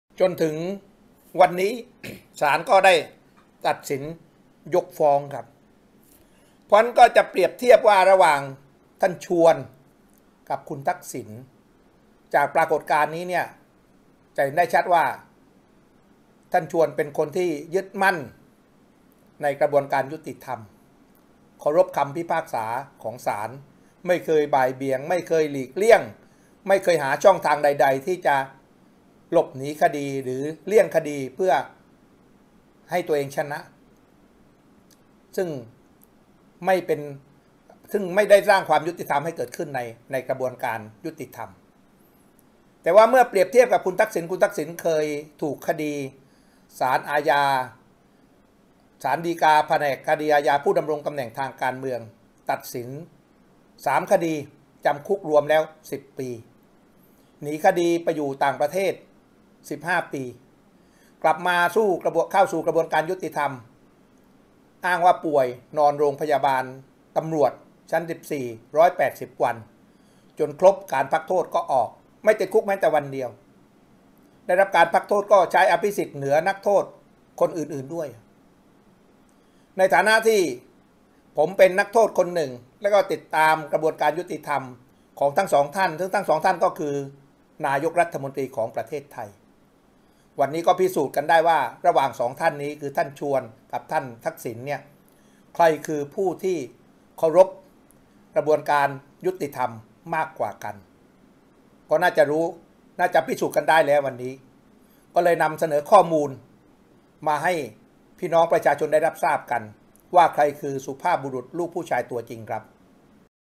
"เทพไท" เทียบ "ชวน-แม้ว" 2 อดีตนายกฯ ใครเคารพกระบวนการยุติธรรมมากกว่ากัน
อดีตส.ส.ปชป. เทียบ “ชวน-ทักษิณ” 2 อดีตนายกรัฐมนตรี ใครคือผู้เคารพกระบวนการยุติธรรม มากกว่ากัน? คนหนึ่งเคารพคำตัดสินของศาล ไม่บ่ายเบี่ยง อีกคนหลีกเลี่ยงคดีความ ไม่ยอมติดคุกแม้แต่วันเดียว